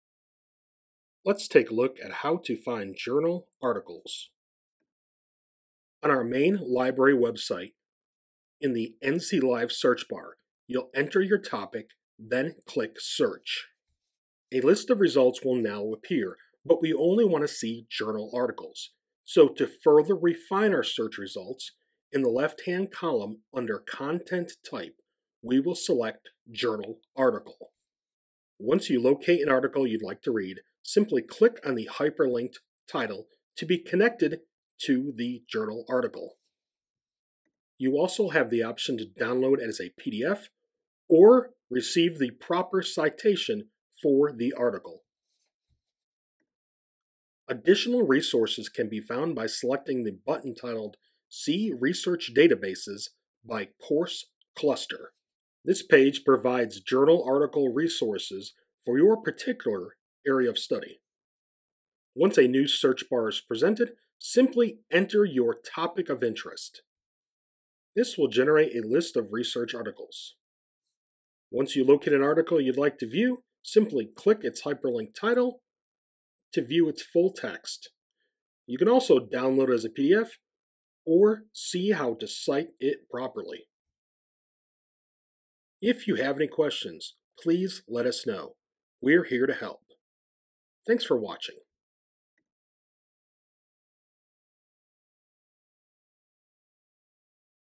How to find articles in the HCC library
Some good tips to help you easily find articles in the HCC library.